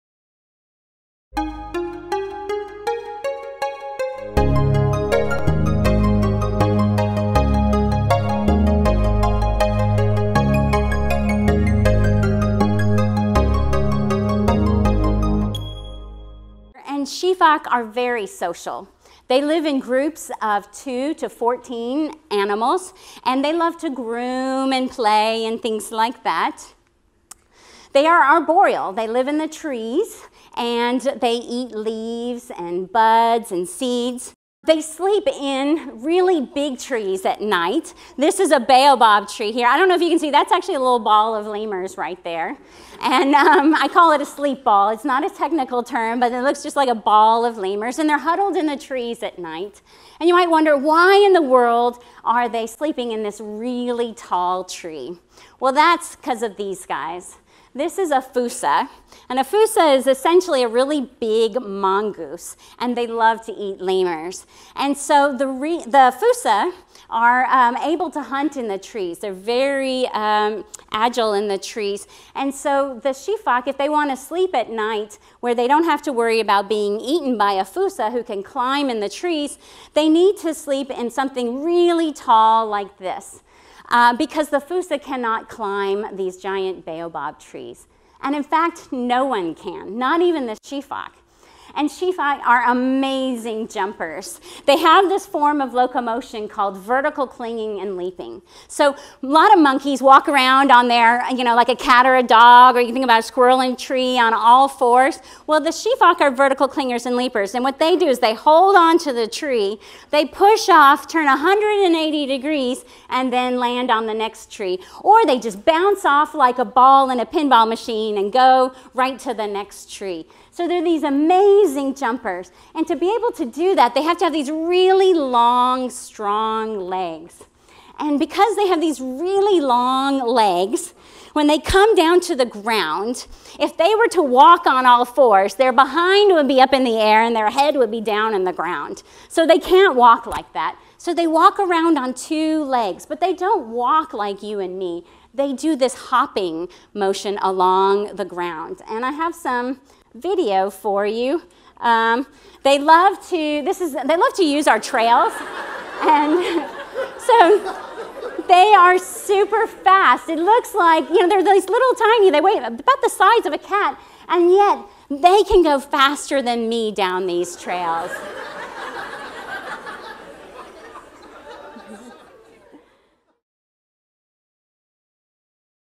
What are Lemurs Like? (HSCT #108)
Hot Science - Cool Talks #108
Dr. Rebecca Lewis

Presented by the Environmental Science Institute as part of Hot Science - Cool Talks.

Discover the secrets behind the enigmatic lemurs!

for more resources and videos.